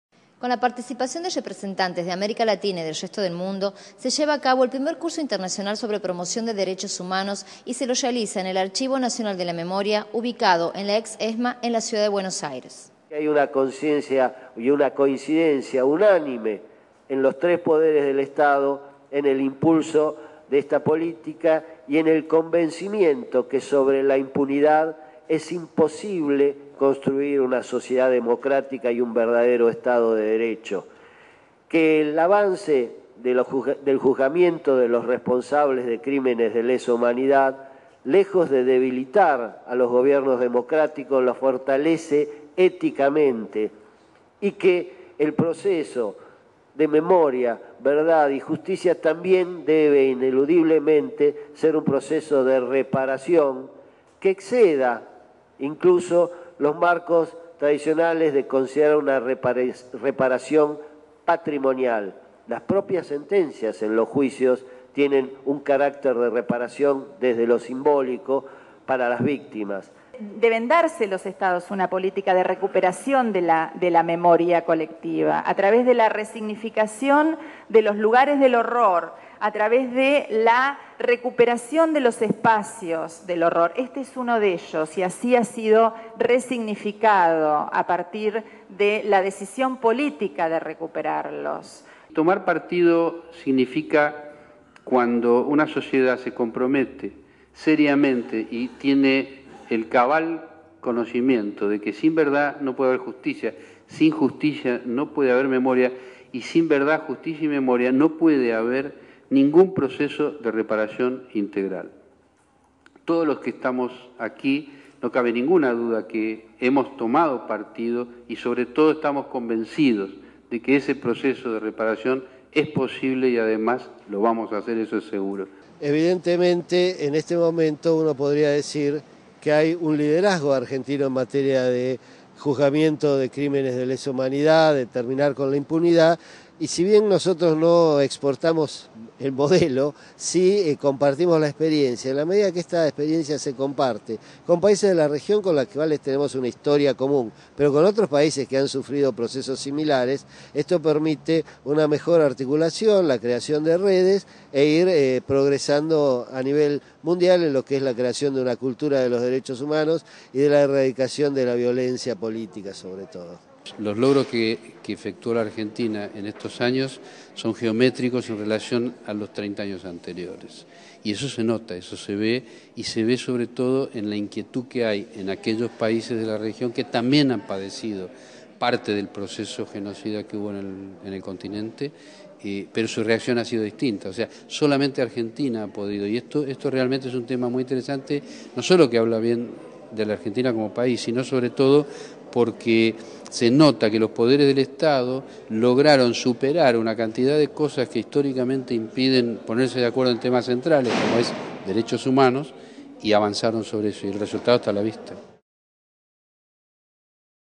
Alén: "Argentina es líder en juzgar crímenes de lesa humanidad"
"Argentina es líder en juzgar crímenes de lesa humanidad", aseguró el subsecretario de Protección de Derechos Humanos, Luis Alén, al participar del Primer Curso Internacional sobre Promoción de los Derechos Humanos.
En su alocución, que tuvo lugar en el Centro Cultural de la Memoria, en la ex Esma, el funcionario subrayó, además, que "hay una coincidencia de los tres poderes del Estado: es imposible construir una sociedad democrática sobre la impunidad".
Por su parte, la directora de Asuntos Jurídicos de la Secretaria de Derechos Humanos, Andrea Gualde, subrayó la importancia de "resignificar los lugares del horror" como política de Estado para recuperar la memoria colectiva.
Carlos Rozanski, presidente del Tribunal Oral Criminal Federal nº1 de La Plata, quien también formó parte del panel, estimó que "los logros que efectuara Argentina en estos años son geométricos en relación con los 30 años anteriores, y eso se nota", enfatizó el magistrado.